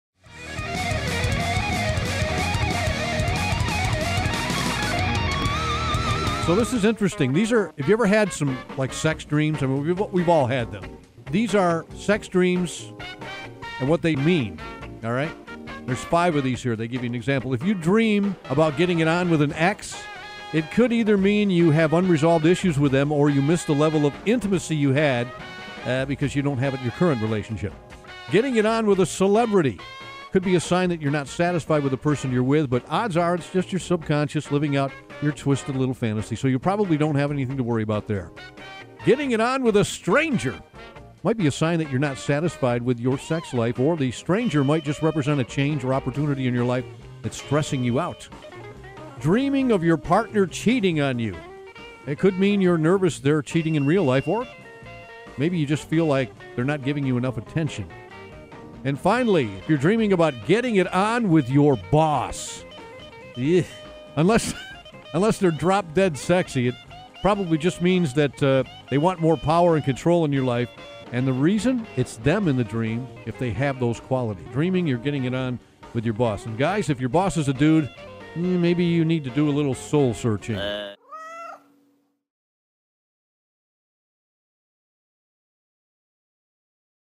What Your Sex Dreams Really Mean - Doug's Morning Shorts
We have all had sex dreams and even though some admittitly are quite pleased, some can make us wake up wondering what the hell? Doug sheds some light on what your sex dreams mean on this edition of Doug's Morning Shorts.